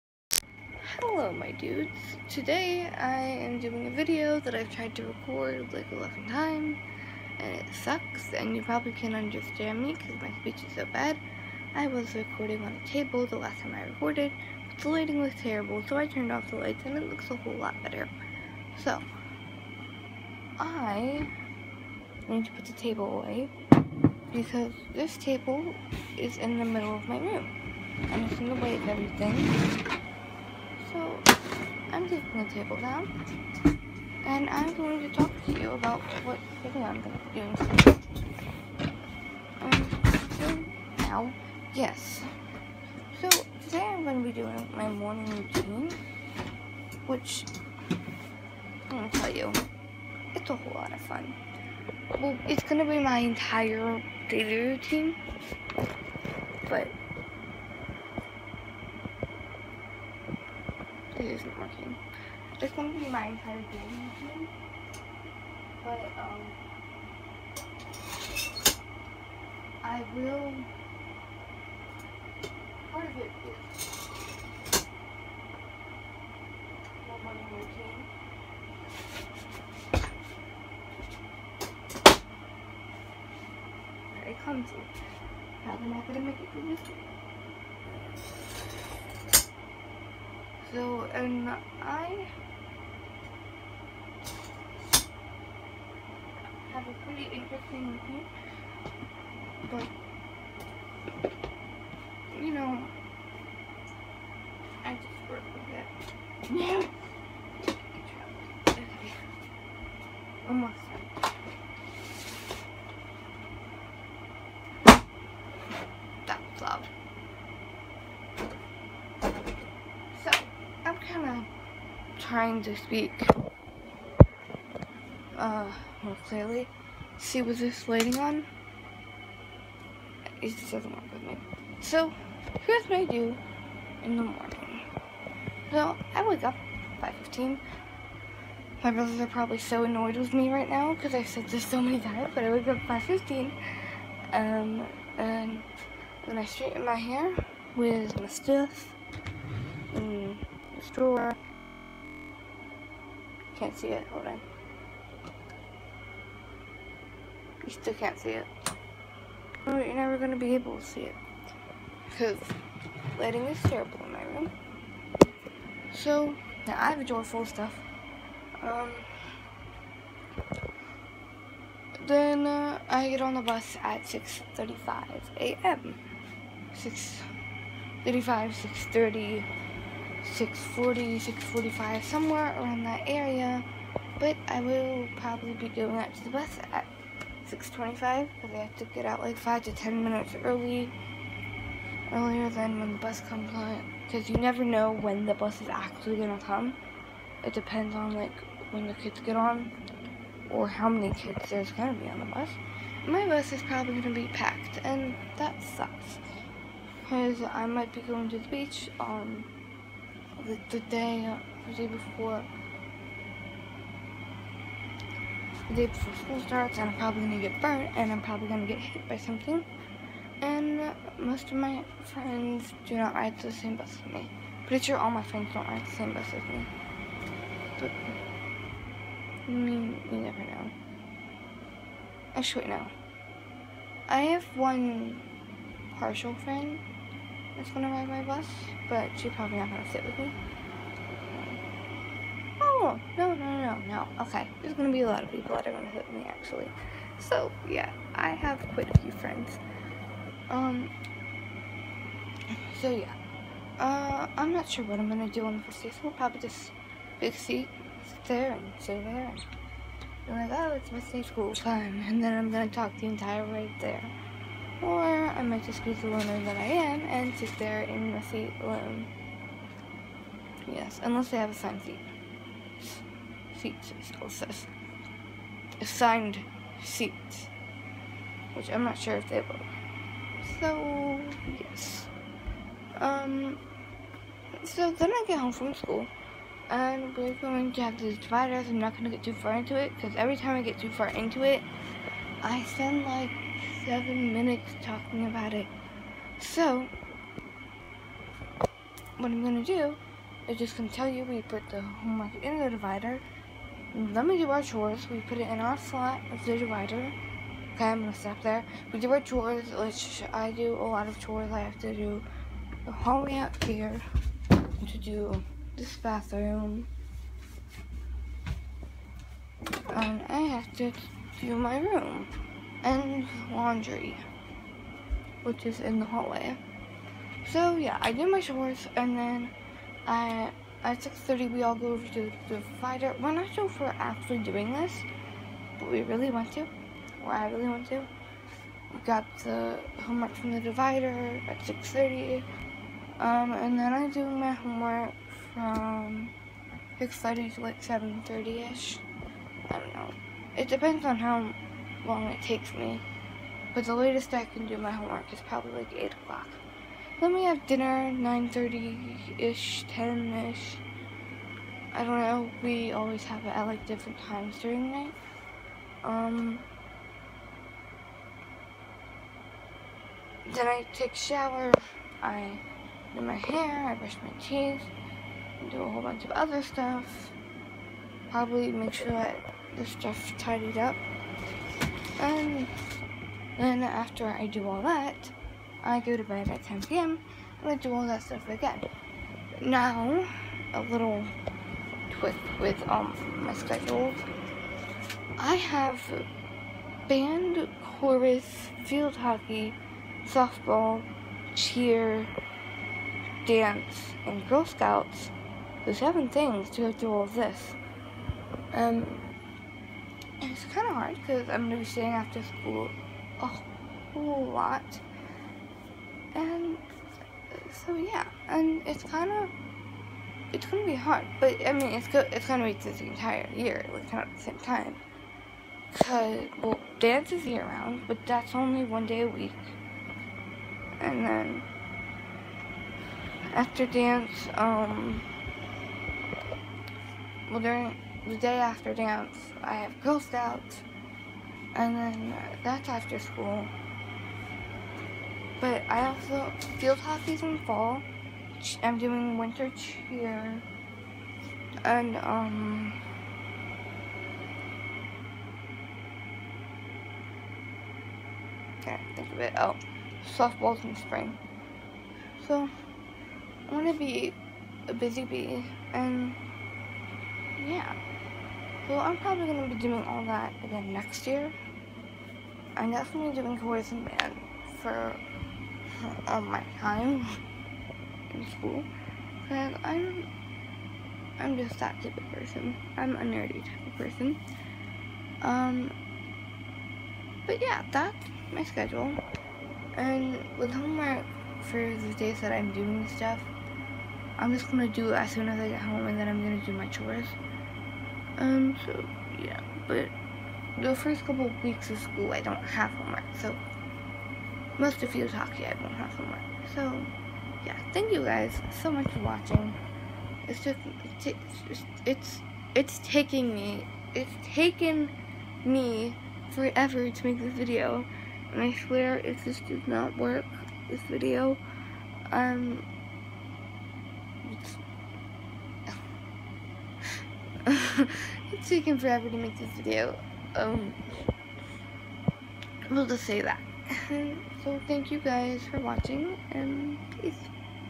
MY SCHOOL DAILY ROUTINE (sry about me moving the camera so much 😂)
Hey my dudes! I am showing you what my morning routine will look like for sixth grade. Even though school hasn’t started yet, me and my little sister made our routines ahead of time so we knew what to do. Hope you enjoy!